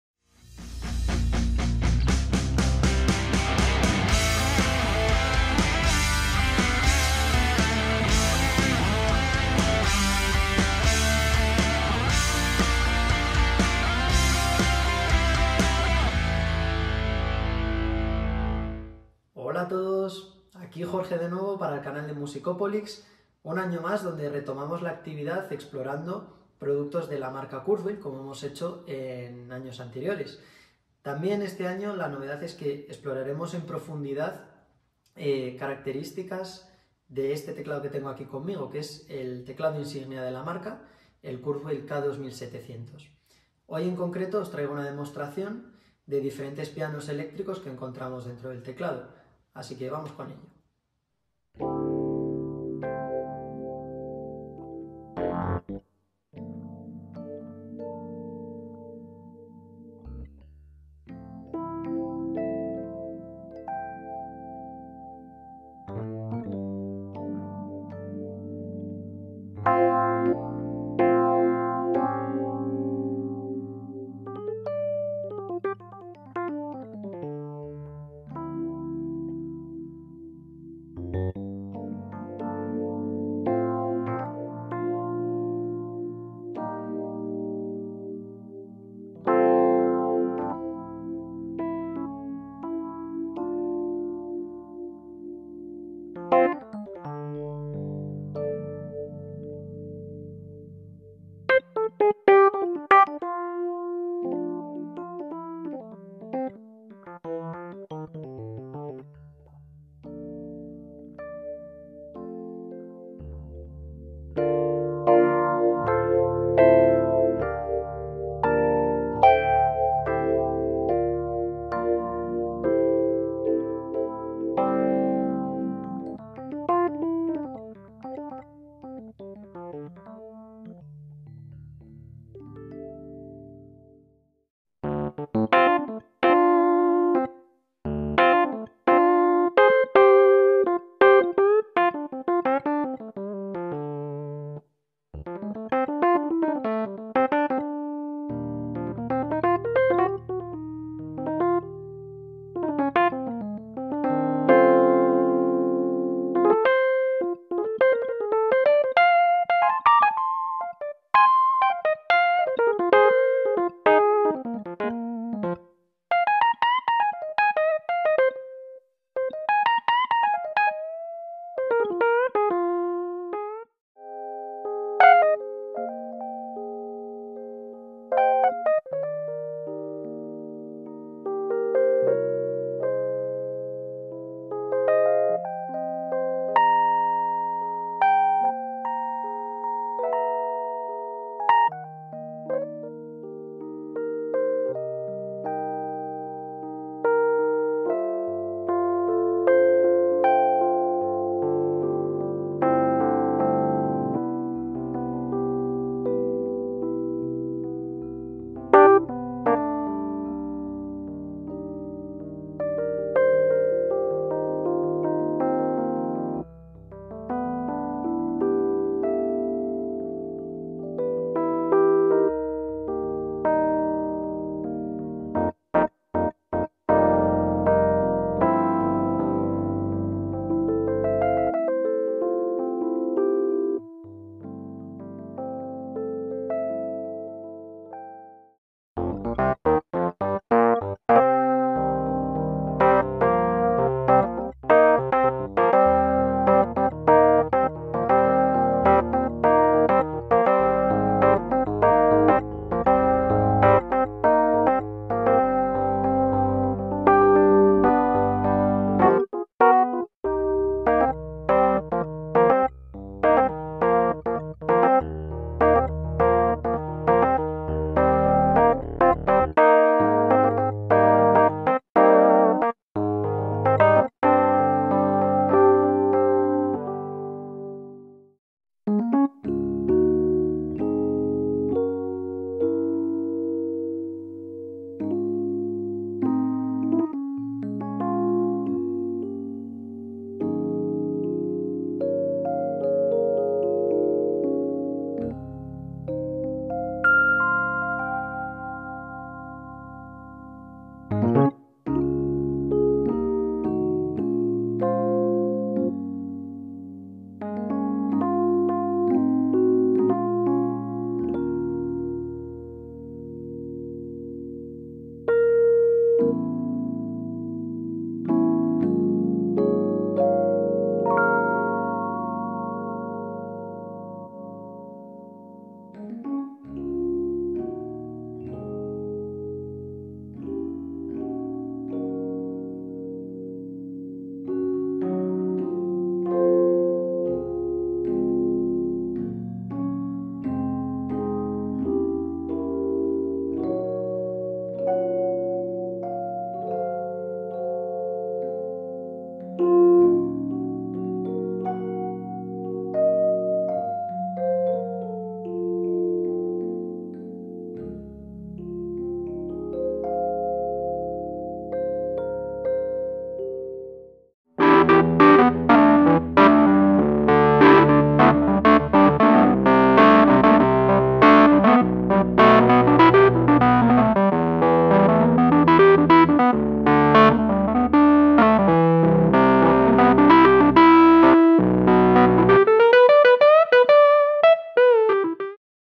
Descubre los sonidos del nuevo Kurzweil K2700 🎹 | Musicopolix
Estamos de vuelta una vez más de la mano de nuestro amigo y artista Kurzweil Jorge para mostraros la amplia gama de productos de la marca, empezando nada más y nada menos que con una demo de sonidos del nuevo piano Kurzweil K2700 🎹💥